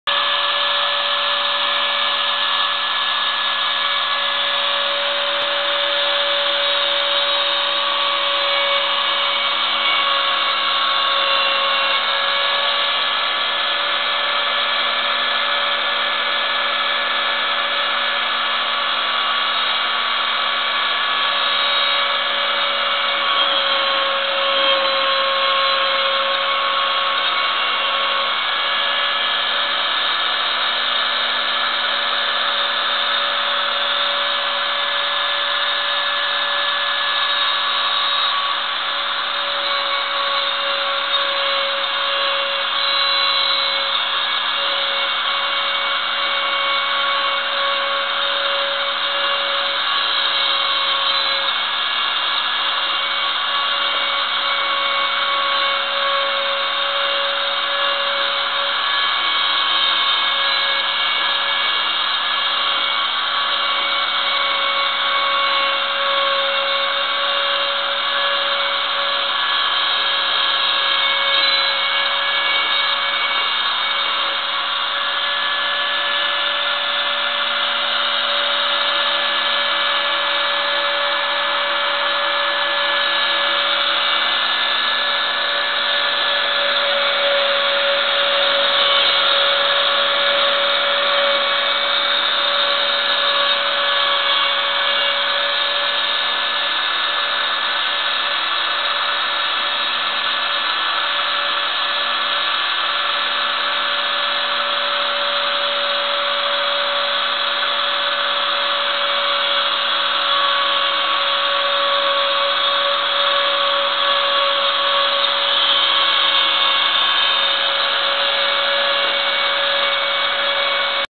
Ooznest Ox going well...
A short video showing my recently built Ox CNC router,  showing a name plaque for a mate's grandson's bedroom door. Big thanks to Darren at WeDo 3D printing, who supplied stepper motors and wheels and to  Ryan and Ryan at at Ooznest for their top quality products and outstanding after sales support and their paitence in helping me get going...